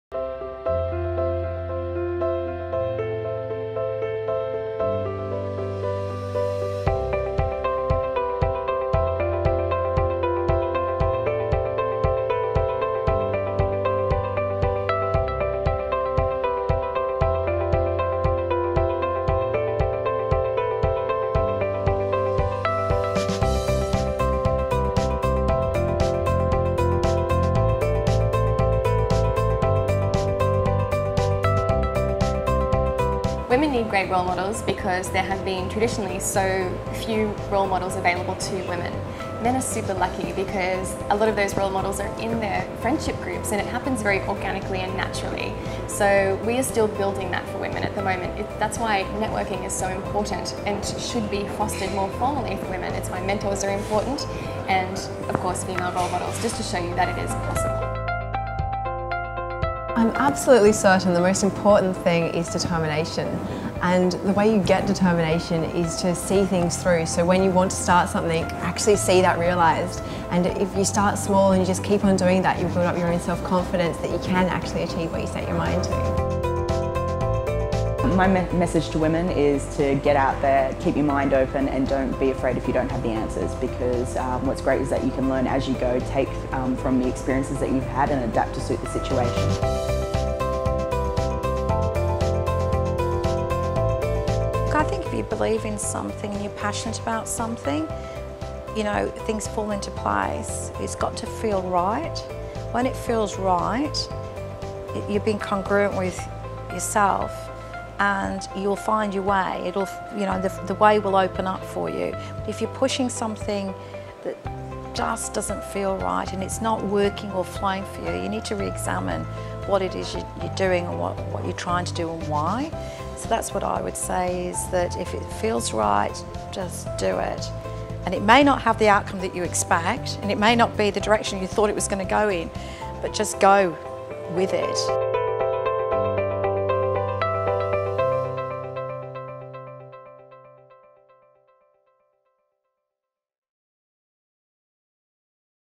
Campaign: International Women’s Day 2015 WeAreThe51 (April 2015)
wearethe51: women make up 51 per cent of the population in developed countries. Watch exclusive (inspirational) behind-the-scenes footage of our wearethe51 campaign in our April 2015 issue.

ENTER TO WIN: As part of this campaign, we are offering readers a unique opportunity to have a personal one-off mentoring session with one of the inspiring women who featured in our April issue. You could be matched with either Julie Bishop, Jodie Fox, Fiona Lowry, Gillian Armstrong, Gai Waterhouse, Izzi Manfredi, Melanie Perkins or Genevieve George.